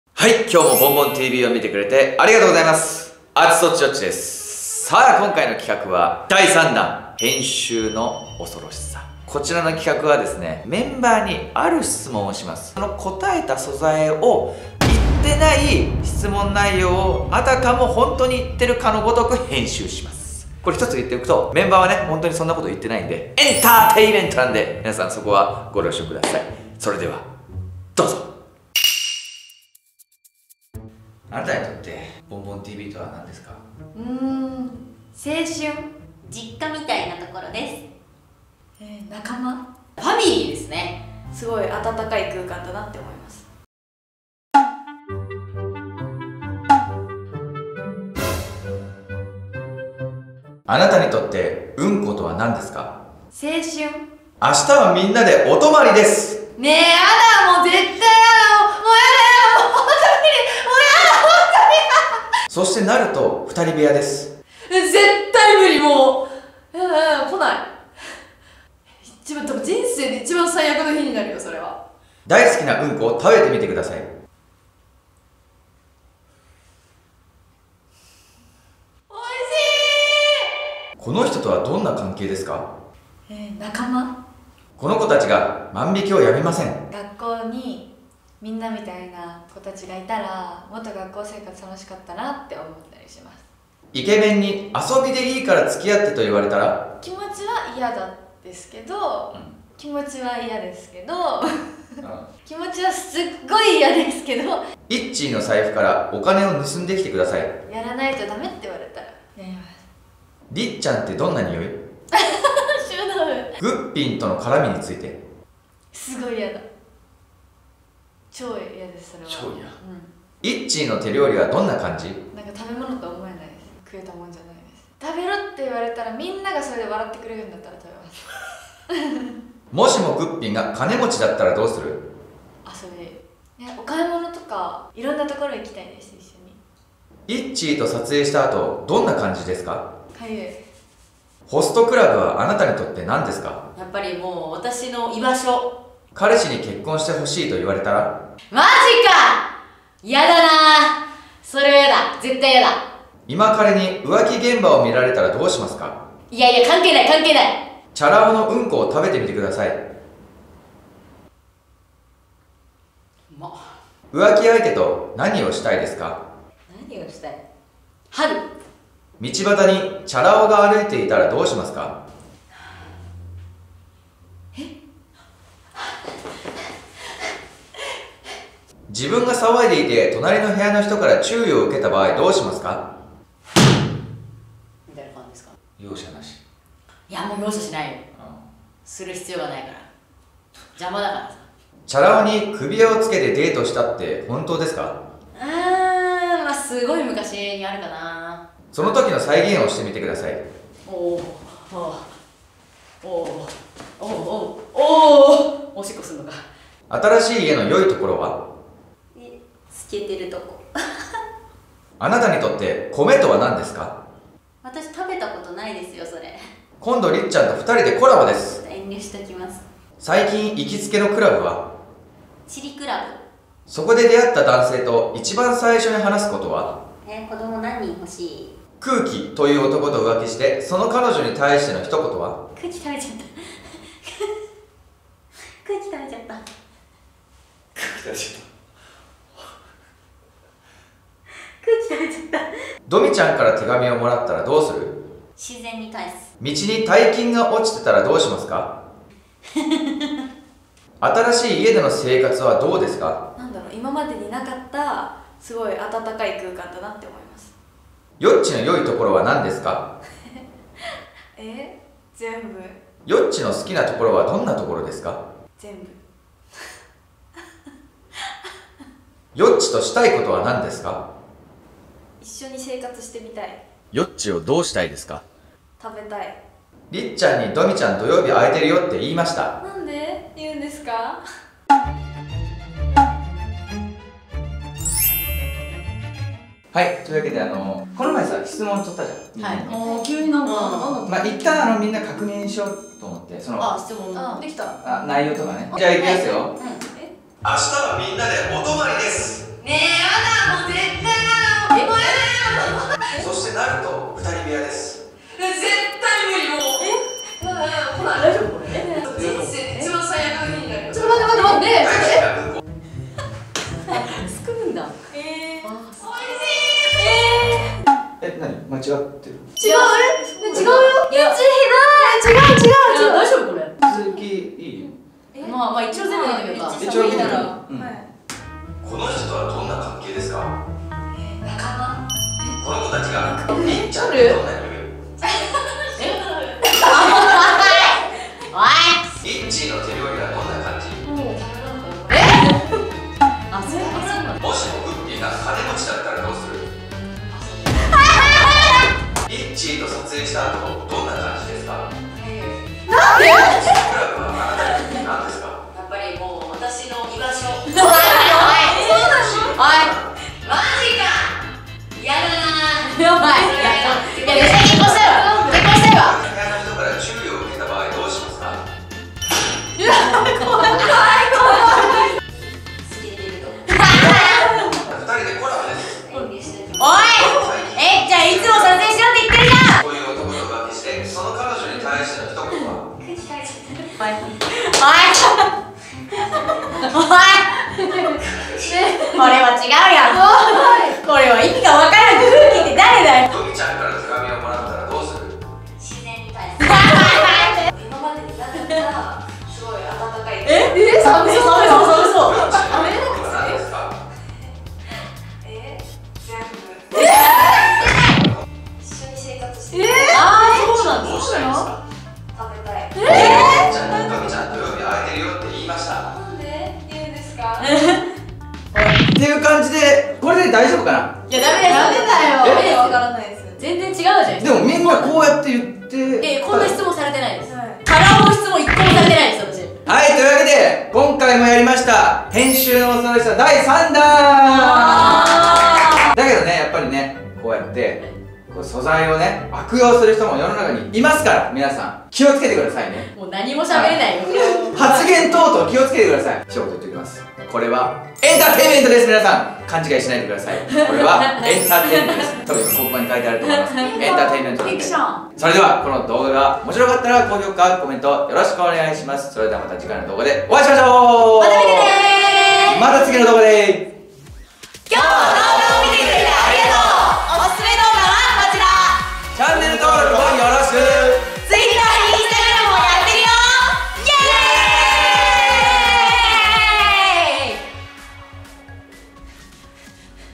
【閲覧注意】実は〇〇の事嫌い！？質問コーナーやったら衝撃の事実が発覚しました…【不仲説】
▽私たちに実験・検証して欲しいネタはこちらのフォームで送って下さい！

りっちゃんです！
久しぶりによっちにやられました…
女子メンバーはみんな仲良しです！
18時にちゃんとした質問コーナーの動画が上がるのでそちらもチェックしてみてね！

【この動画を見た人にオススメの動画！】

コミック【ボンボンTVのユーチューバーな日常】第１巻が